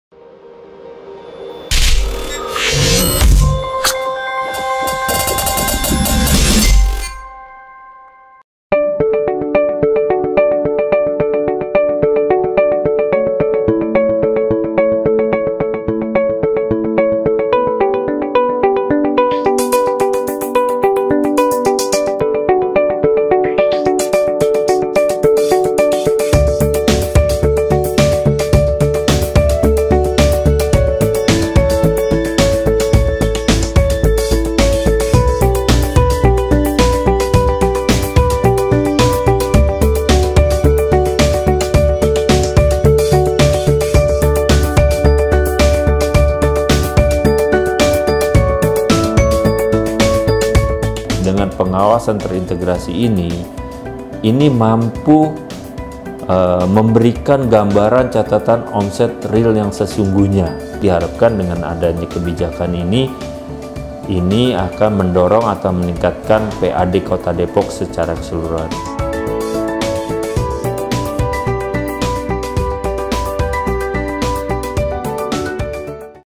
BKD Permudah Pembayaran Pajak Lewat Program Easy Tax
berita.depok.go.id- Badan Keuangan Daerah (BKD) Kota Depok terus menciptakan inovasi-inovasi untuk memudahkan Wajib Pajak (WP) dalam membayar pajak. Salah satunya melalui program Easy Tax yang menyediakan berbagai pilihan, yaitu akses pendaftaran wajib pajak, pelaporan omzet pajak, serta pembayaran pajak.

“Easy Tax ini juga bisa meningkatkan Pendapatan Asli Daerah (PAD) Kota Depok. Dengan kemudahan tersebut, diharapkan perolehan pajak bisa terus meningkat,” ujar Kepala Bidang Pajak Daerah 1 BKD Kota Depok, Endra, di ruang kerjanya, Rabu (16/06/21).

Endra mengatakan, WP tidak perlu datang ke Kantor BKD untuk membayar pajak. Pembayaran cukup dilakukan melalui aplikasi atau marketplace yang telah disediakan.

“Seperti Tokopedia, Traveloka, Bukalapak, Indomaret, Alfamart. Ada juga melalui bank seperti BJB, BTN, BSM, BNI, CIMB Niaga dan OCBC NISP,” terangnya.

Selain itu, lanjutnya, dalam pengoptimalisasian pendapatan daerah, pihaknya juga meluncurkan alat pencatat omzet dalam jaringan terintegrasi. Dikatakannya, setiap tempat usaha yang memasang alat ini, tarif pajak otomatis dikurangi.

“Untuk restoran dari 10 persen menjadi 7 persen. Dengan penurunan tarif pajak akan mempercepat pemulihan ekonomi daerah serta peningkatan daya saing usaha,” pungkasnya. (VD 02/EVD 01/EUD02)